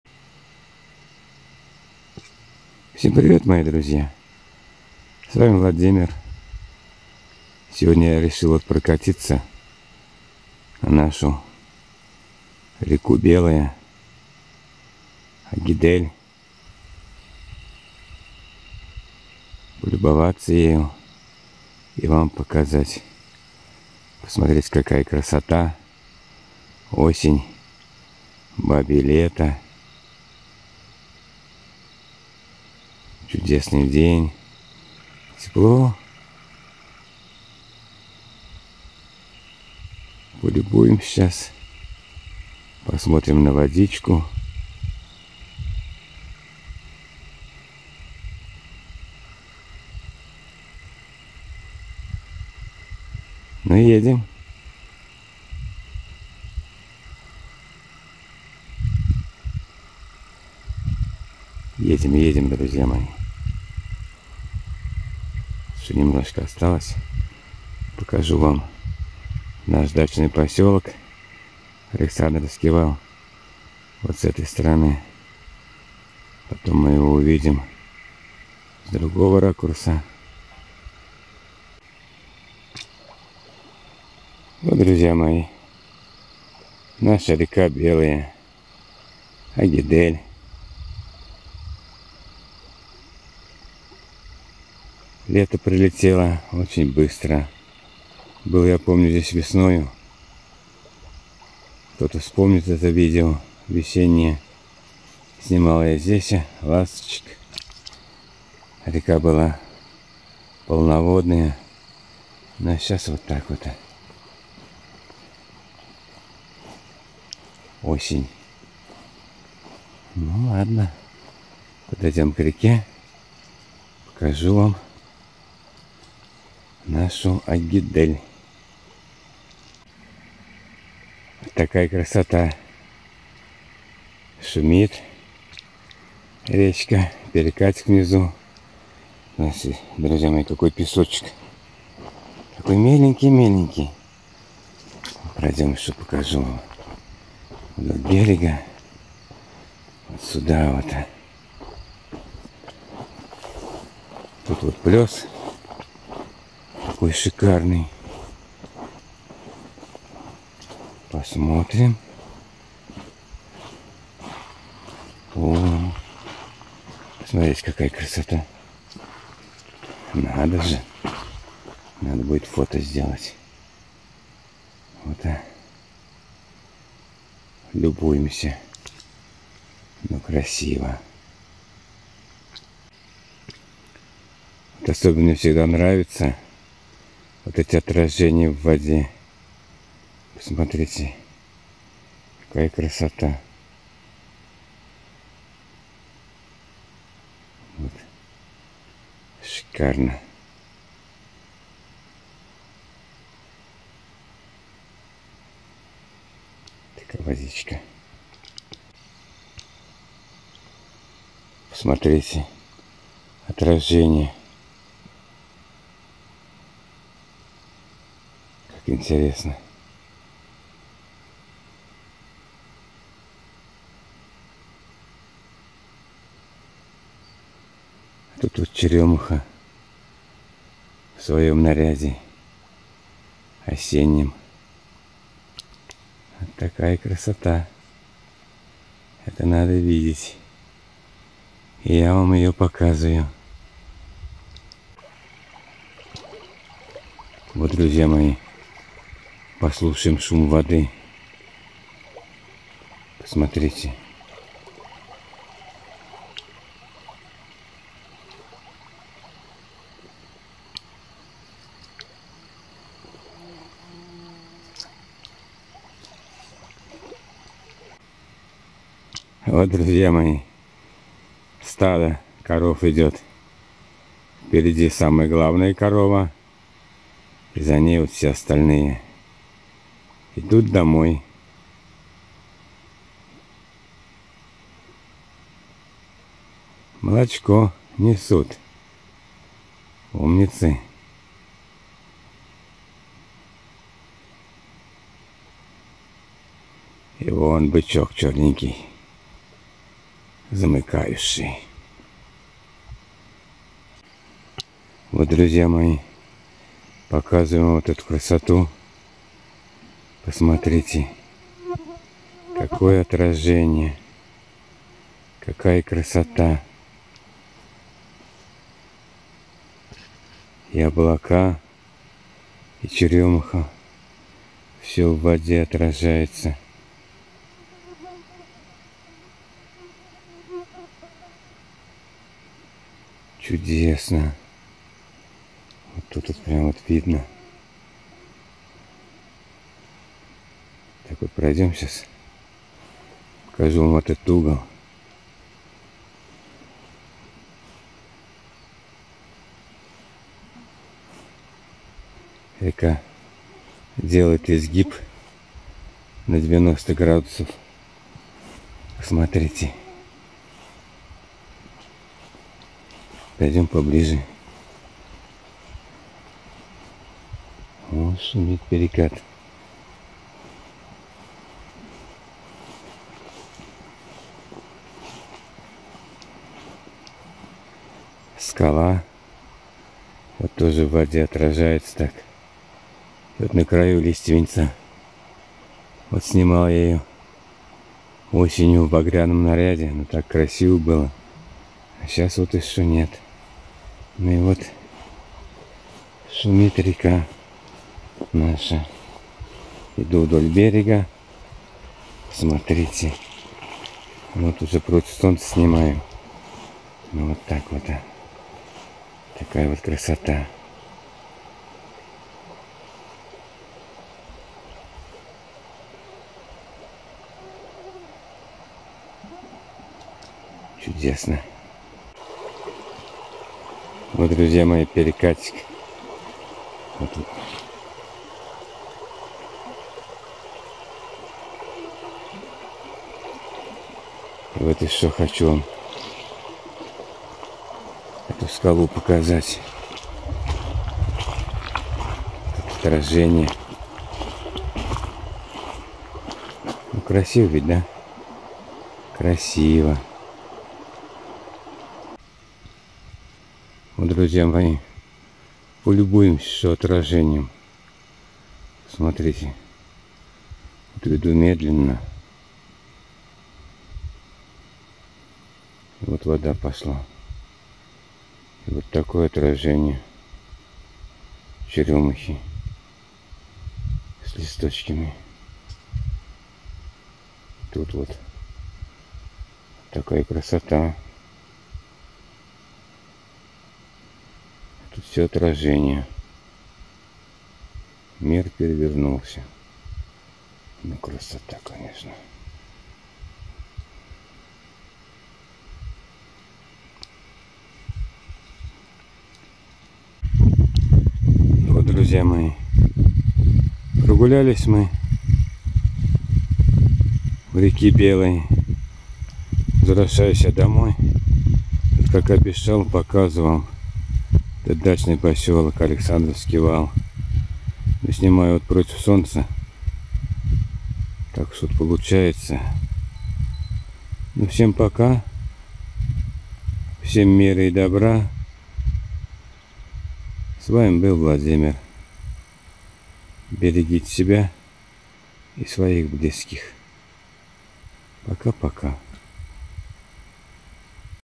Река Белая ! Наша Агидель ! Золотая осень !
Река Белая  , наша красавица Агидель ! Золотая осень у реки Агидель  недалеко  от села Тирлян , у  дачного поселка Александровский вал ! Посмотрите на красоту реки Белой , Агидель !